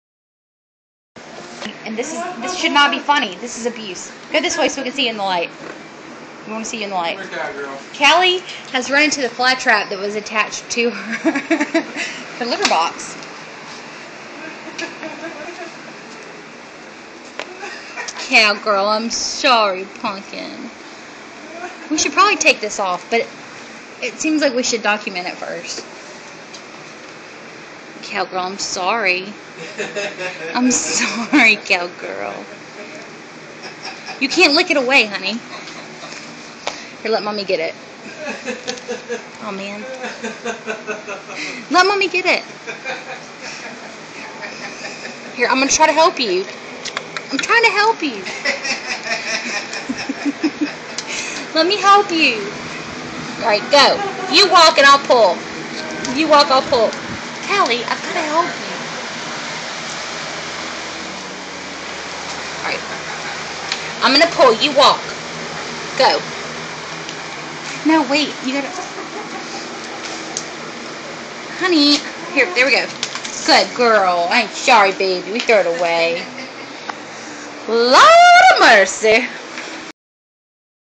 The cat and the fly...trap
Cat gets fly trap stuck across her butt